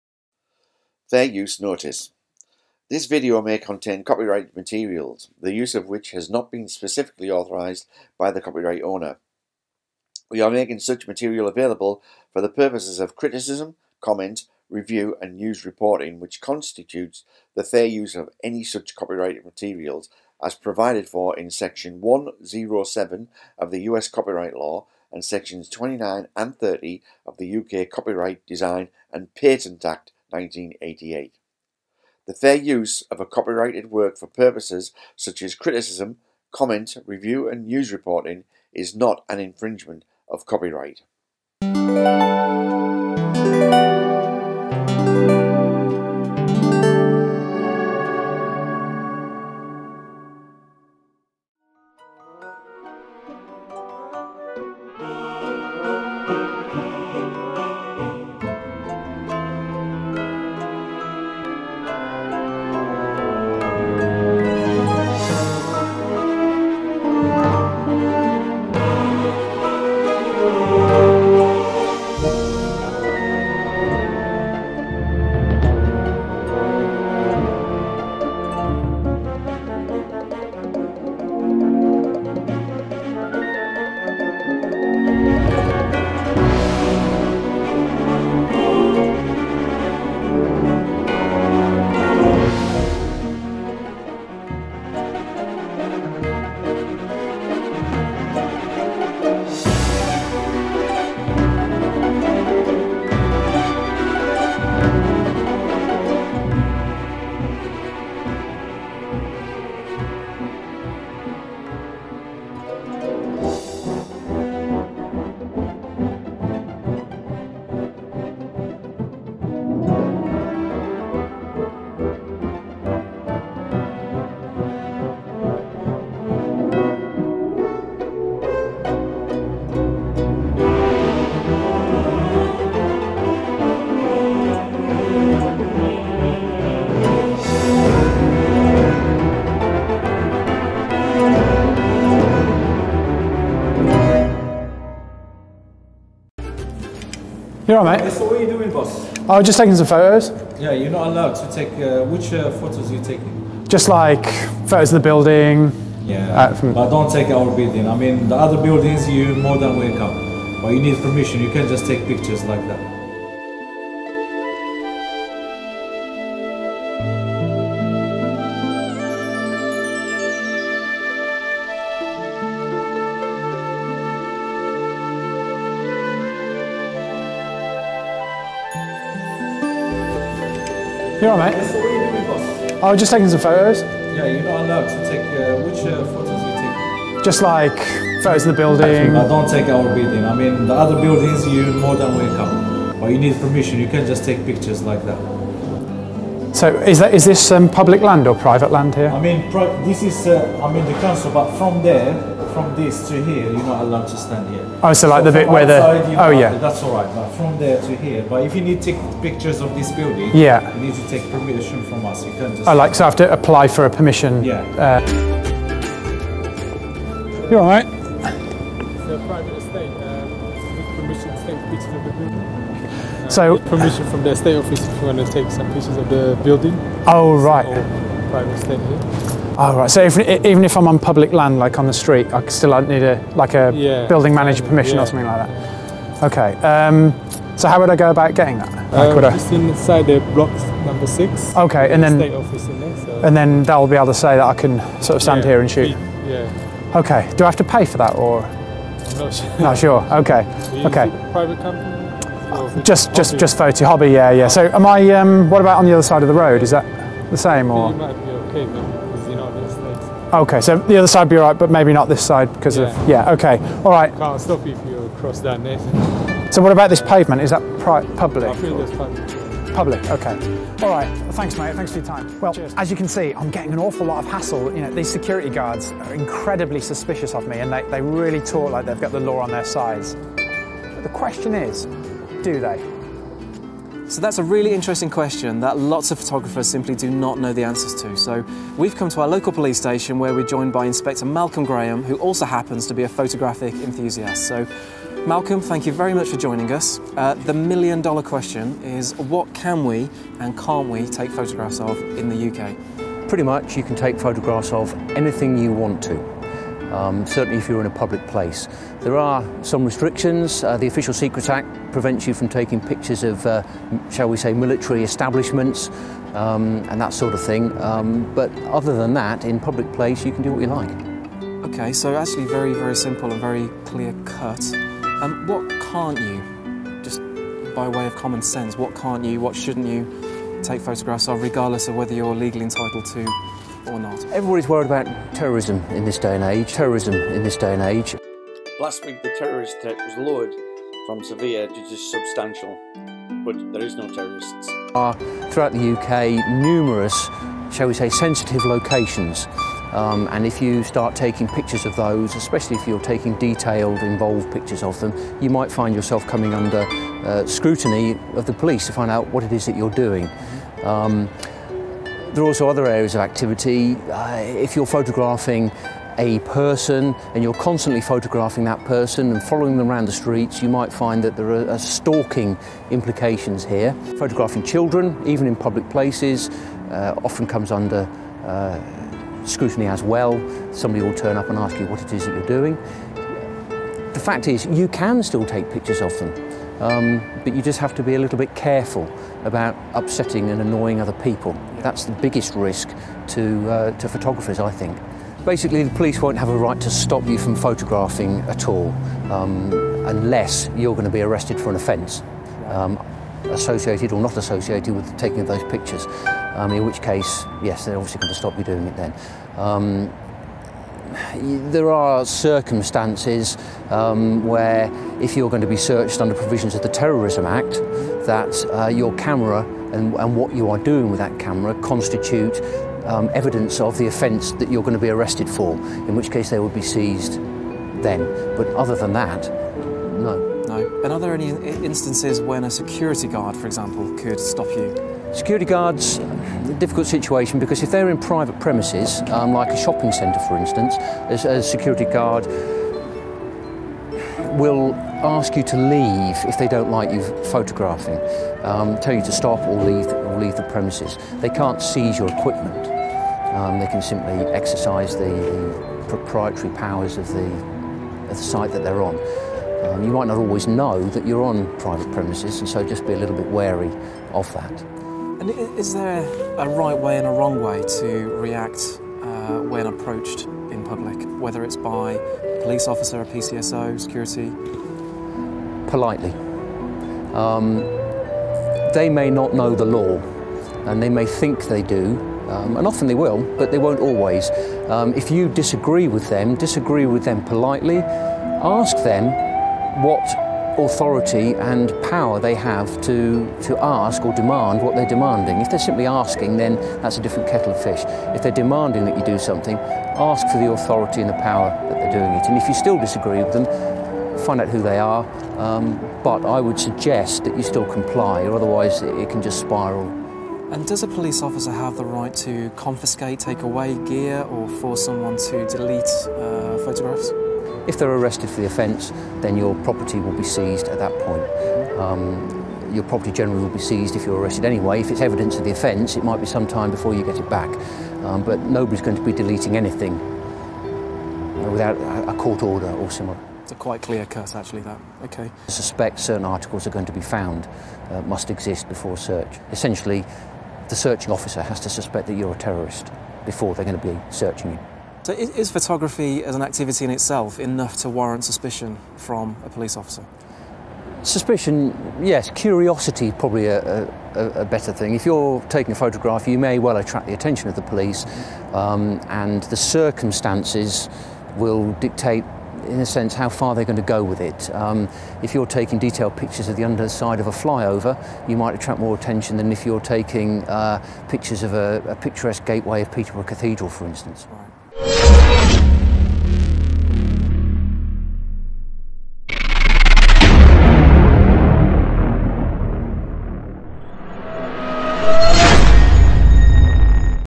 Who says photography is not allowed?
This guy says it’s ok to take photos in public. From the horses mouth and it hasn’t changed!